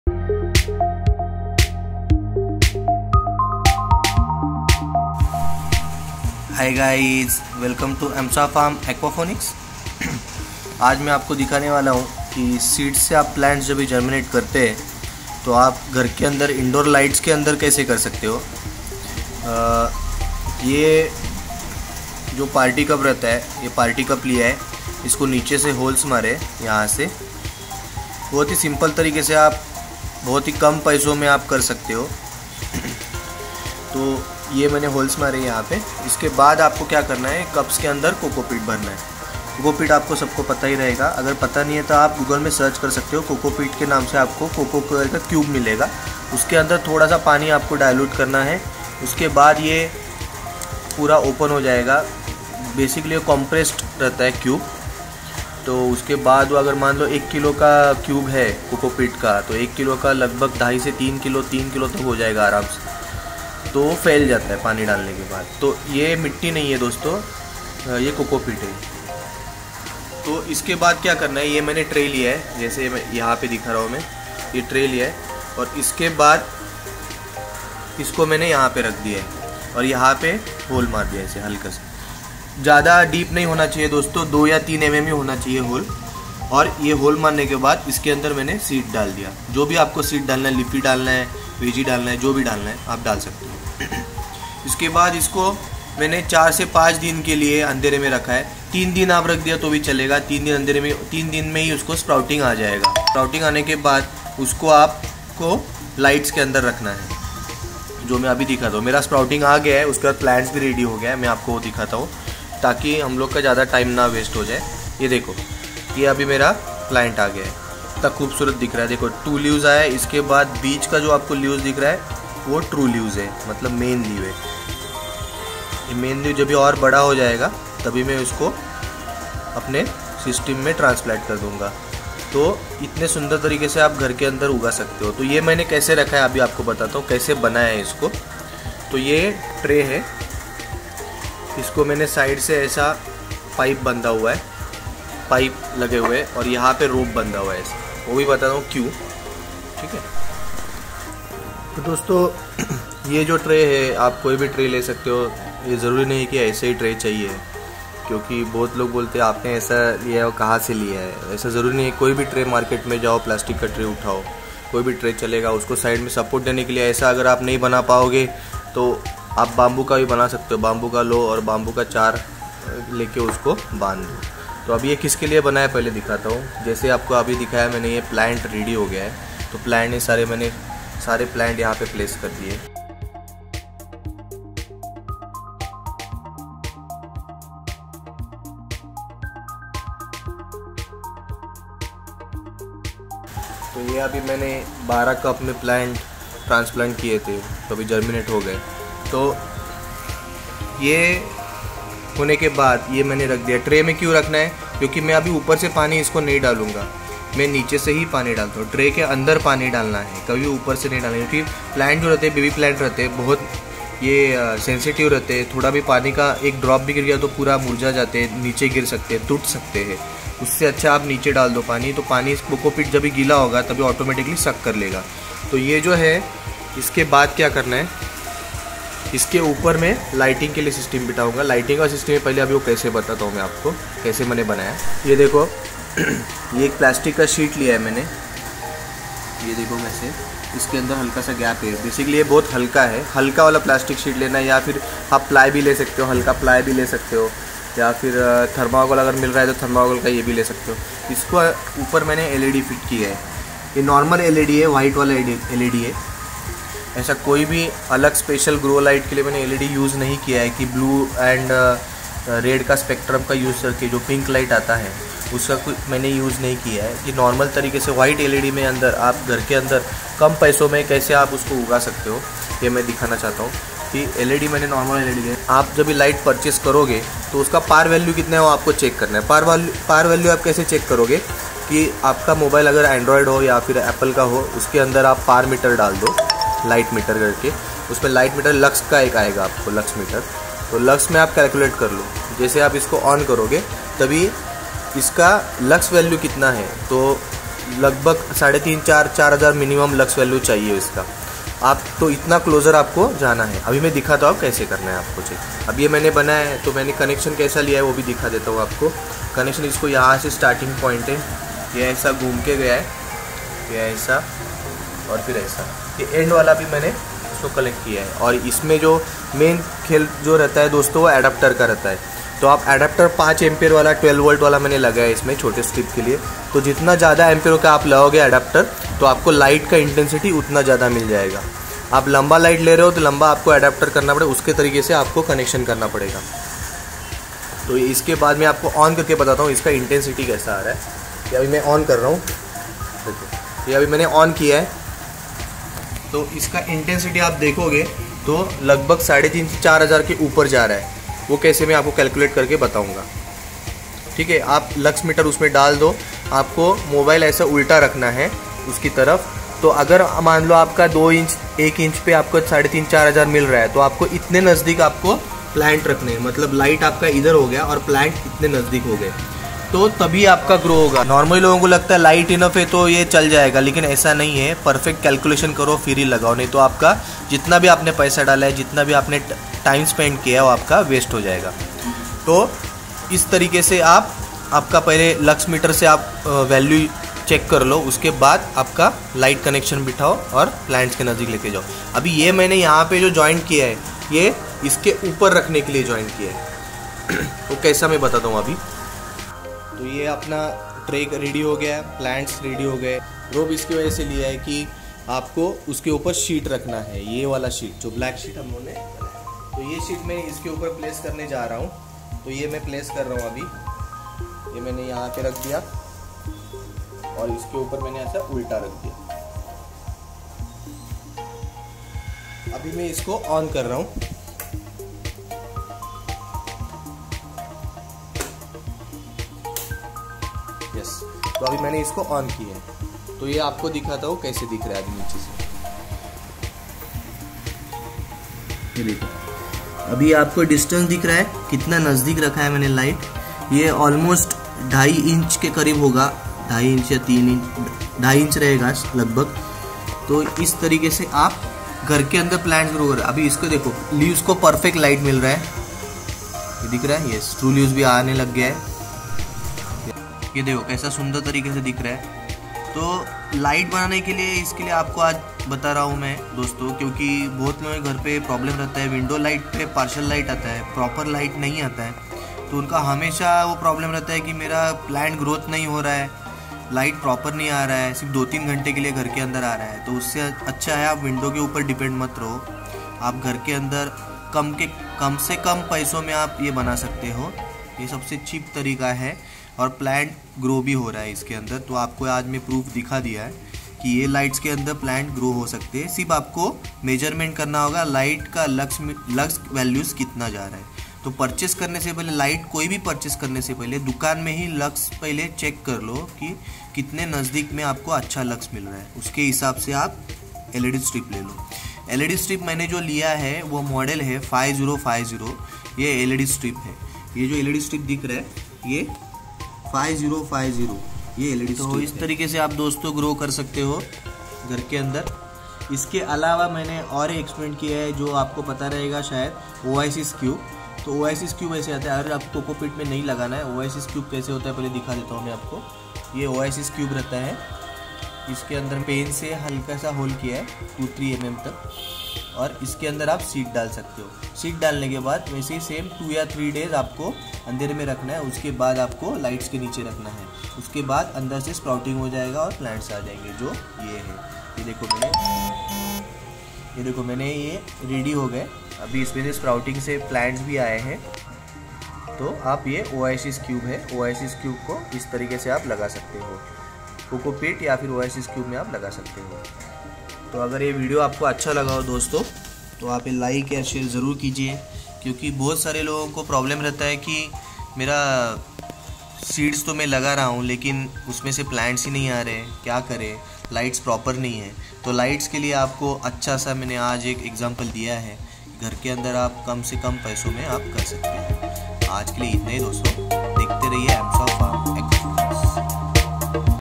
Seed germination below grow lights for indoor growers
Hi, Friends, today will show you how to grow plants or seed germination in indoor with grow lights with small amount of investing.